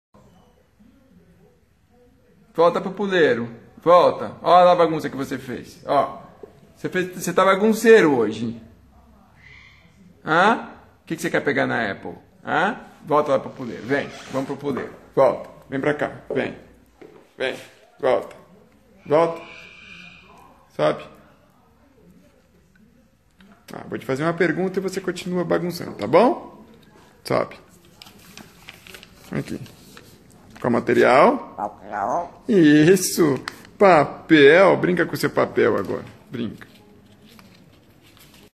Jonathan Bagunceiro
Jonathan fazendo bagunça durante o treinamento...tem dia que ele está com a macaca...